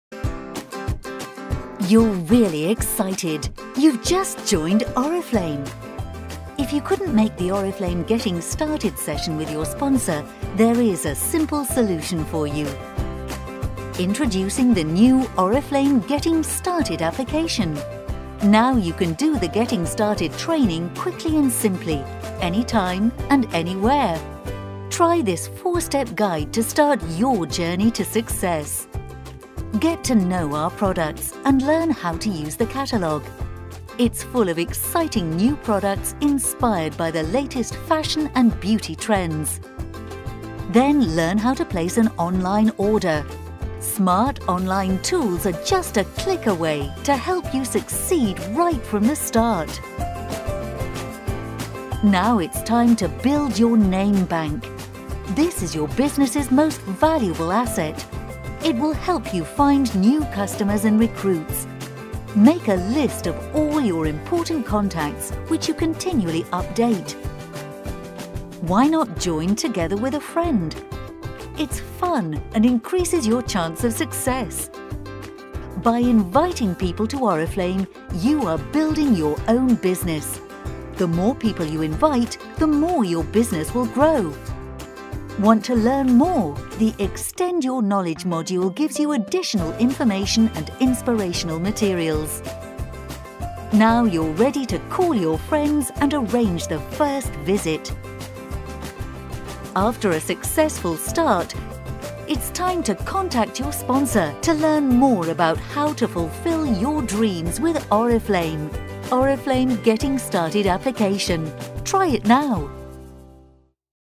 Ứng dụng Khởi Nghiệp cùng Oriflame
Ứng dụng Khởi nghiệp cung cấp cho Tư vấn viên mới mọi thông tin cần thiết để bắt đầu cùng Oriflame! Ứng dụng này sẽ hướng dẫn họ cách sử dụng mạng xã hội và sở thích cá nhân để xây dựng hệ thống kinh doanh thành công và thực hiện ước mơ của mình.